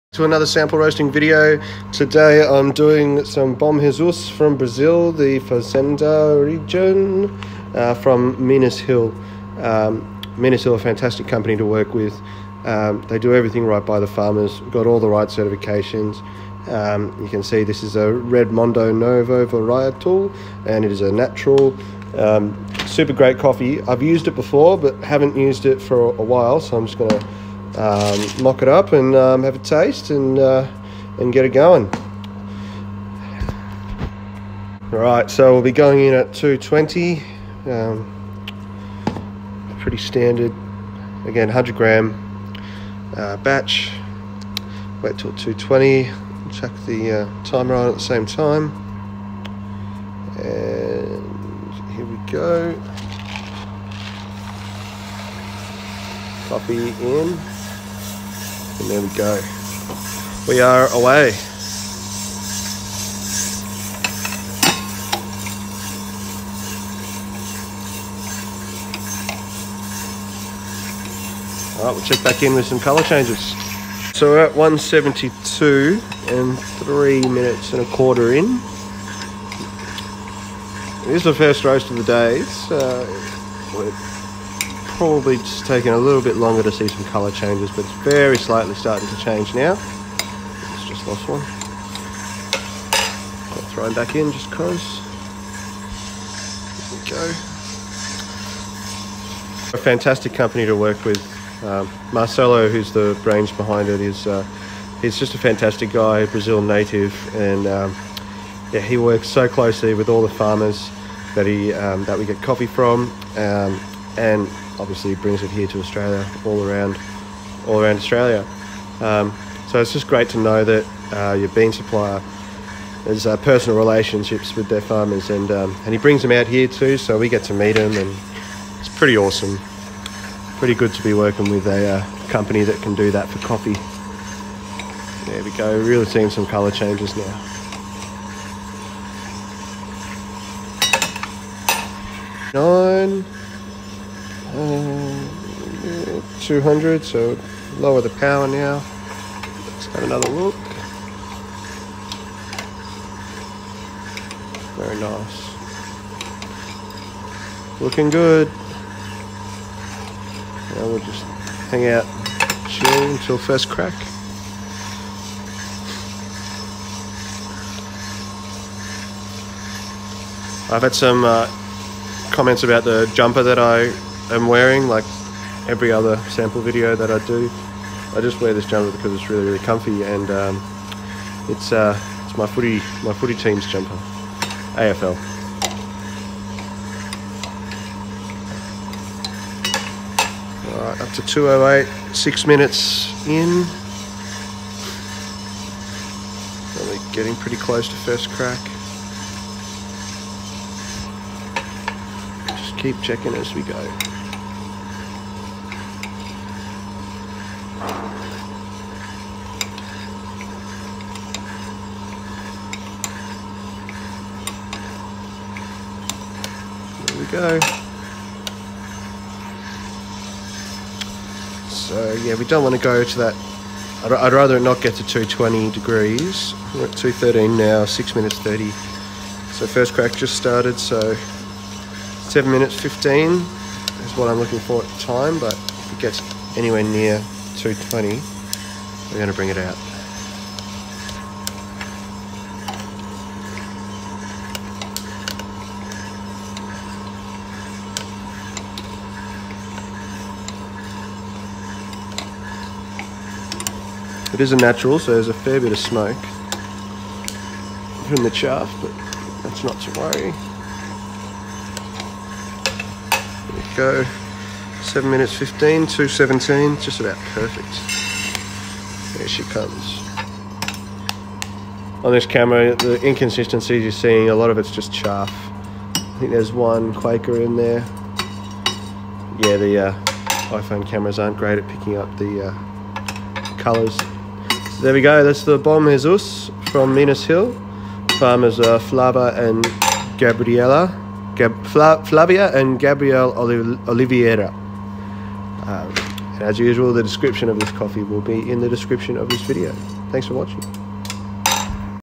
Bom Jesus SAMPLE ROAST
One of the best coffees out of Brazil. Brought to us from Flavia and Gabriel Oliveira.
These 2 farmers do so much for their community In Brazil, often processing other farmers crops just as a gesture of kindness.

Full bodied coffee, cocoa and fresh butter. Creamy aftertaste.